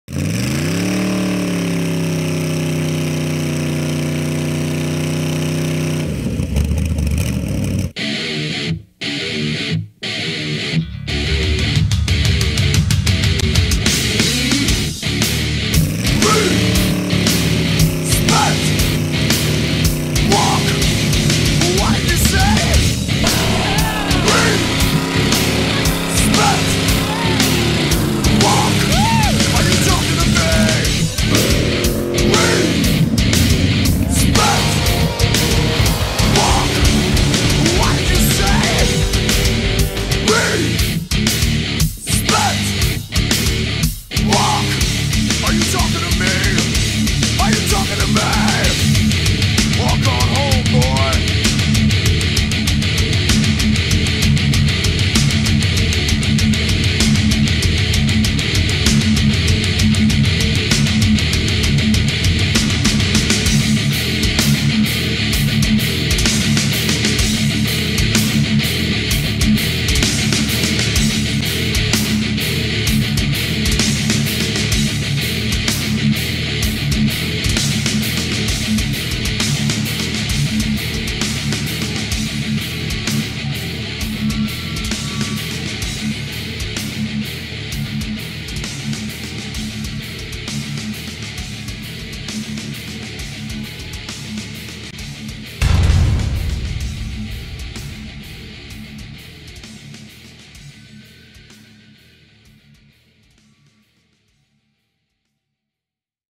V8 Burnouts
Charlie with Backyard Mechanics and Mike Kelly with Team Summit put their bikes to the test with extreme burn-outs.